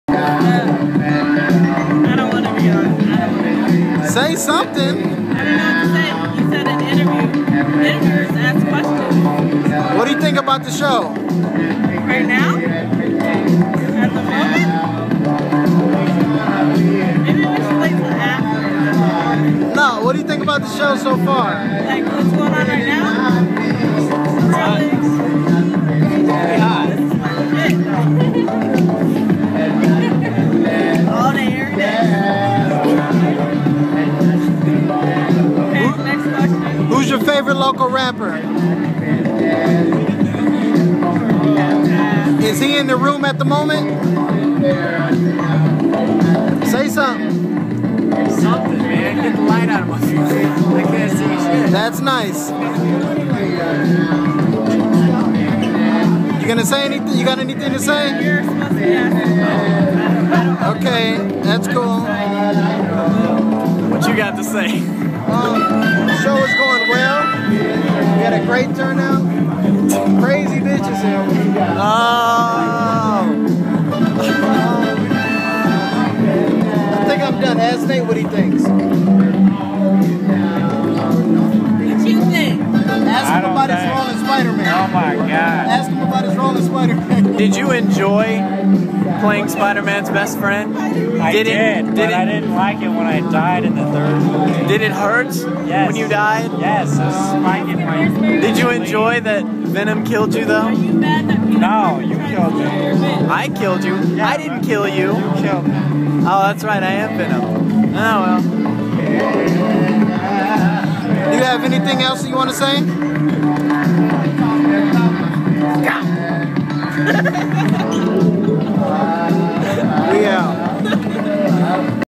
BTS: Interview
Talking with supporters before show